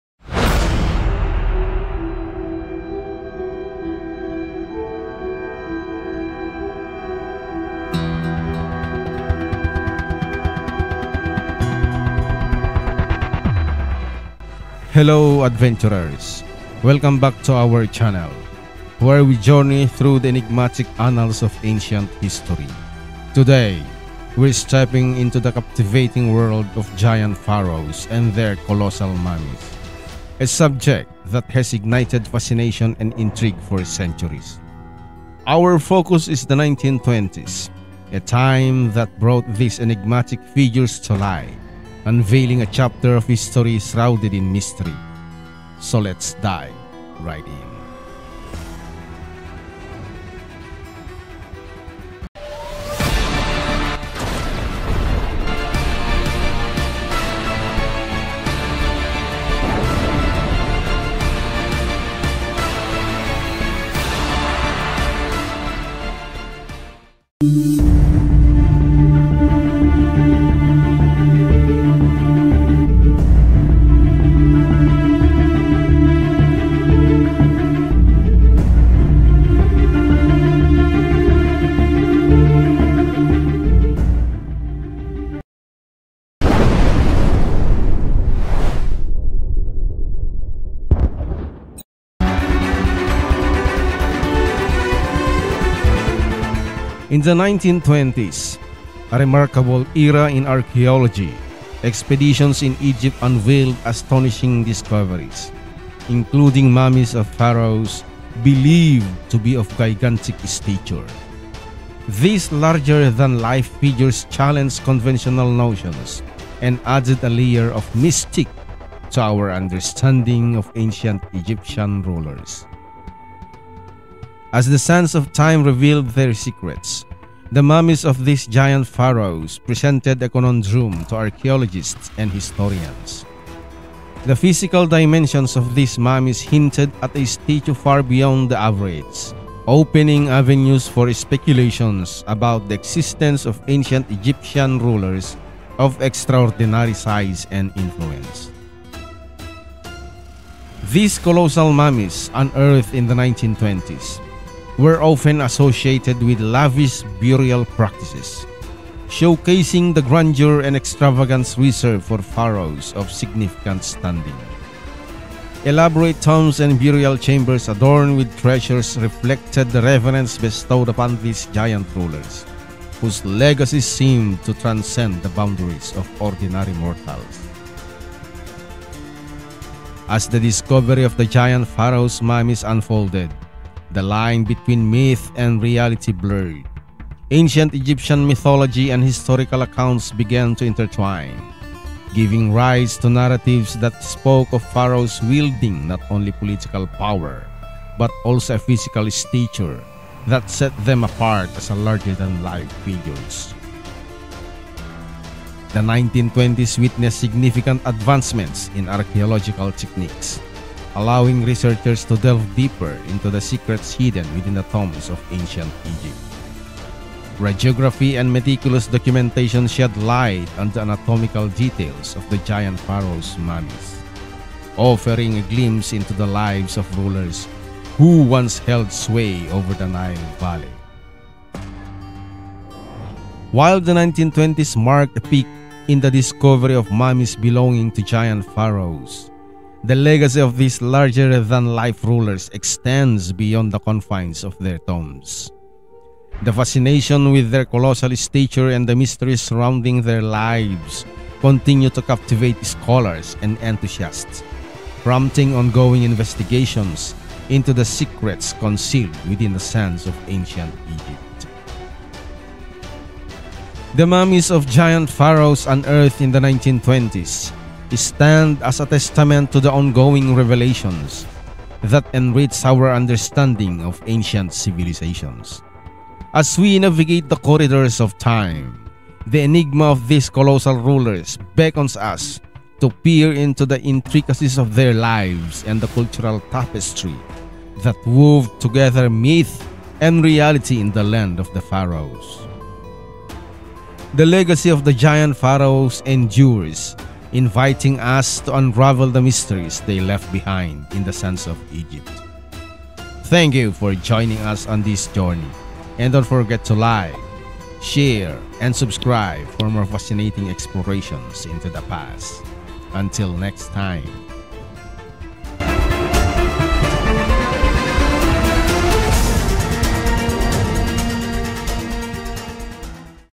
What Howard Carter’s Astonishing Discovery? Giant Pharaohs’ Mummies Unearthed in 1920s
In the 1920s, a remarkable era in archaeology, expeditions in Egypt unveiled astonishing discoveries, including mummies of pharaohs believed to be of gigantic stature. These larger-than-life figures challenged conventional notions and added a layer of mystique to our understanding of ancient Egyptian rulers.
As the sands of time revealed their secrets, the mummies of these giant pharaohs presented a conundrum to archaeologists and historians. The physical dimensions of these mummies hinted at a stature far beyond the average, opening avenues for speculation about the existence of ancient Egyptian rulers of extraordinary size and influence.